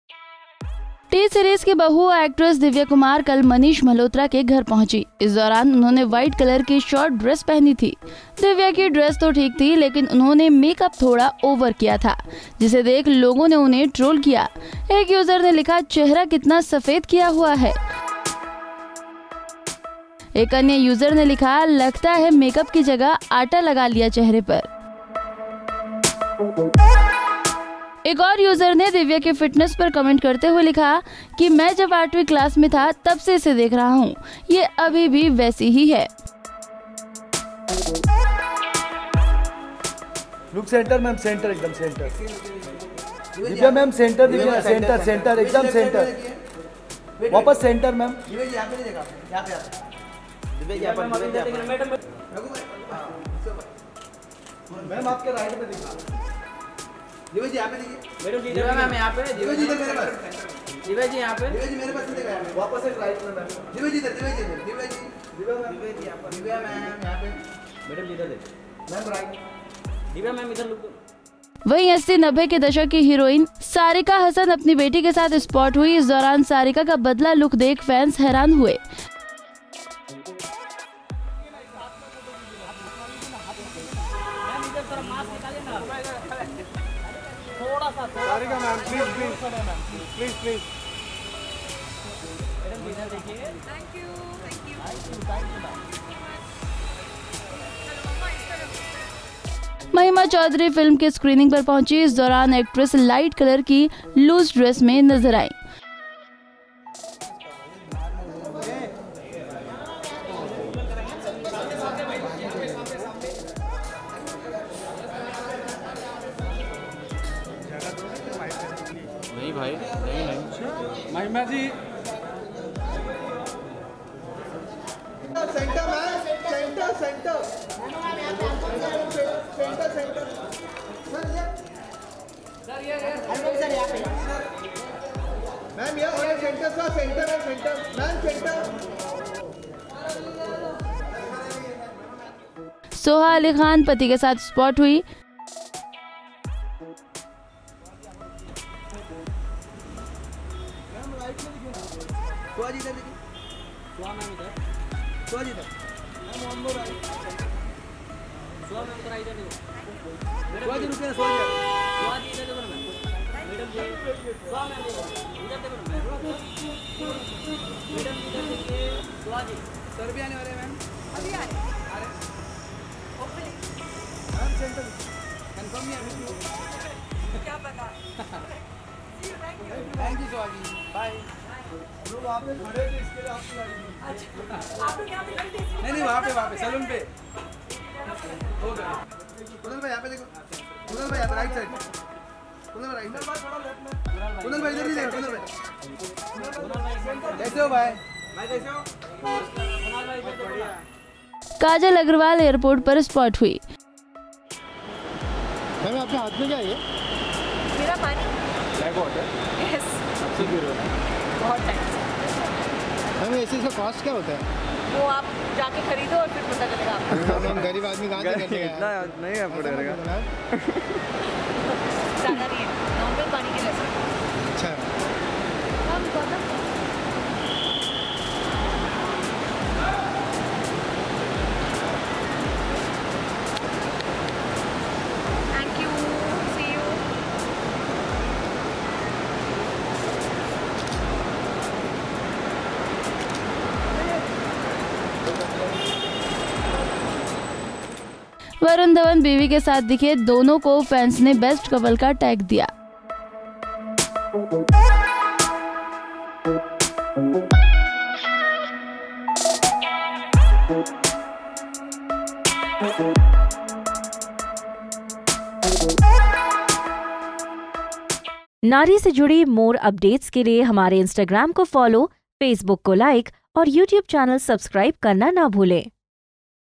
T-Series की बहू मेकअप की जगह आता लगा आई, 90 दश्क की Sarika का बढ़ा हुआ वजन देखकर लोग हैरान
T-Series की बहू मेकअप की जगह आता लगा आई, 90 दश्क की Sarika का बढ़ा हुआ वजन देखकर लोग हैरान, वही Mahima की क्यूटनेस ने जीता फैंस का दिल!